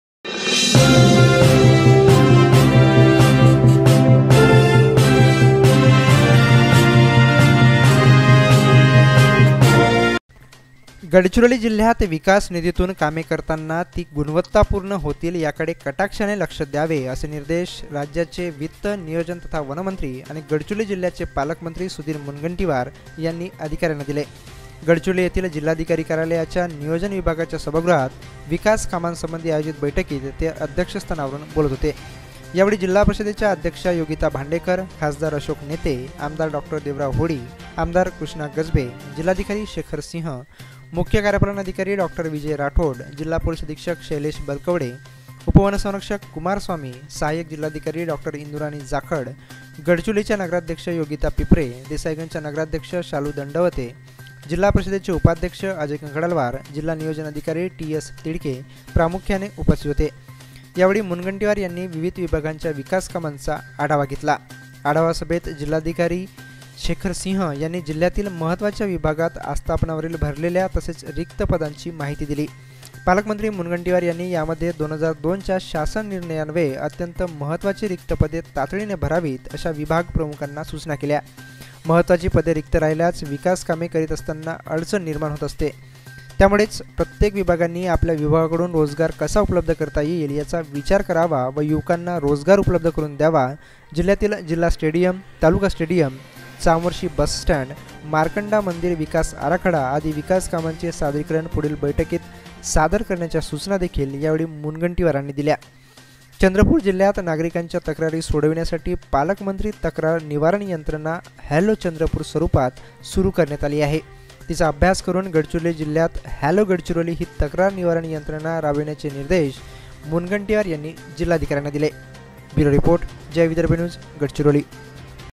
पालकमंत्री तक्रार निवारण यंत्रणा राबविणार: मुनगंटीवार
विकासनिधीतून गुणवत्तापूर्ण कामे करण्याचे दिले निर्देश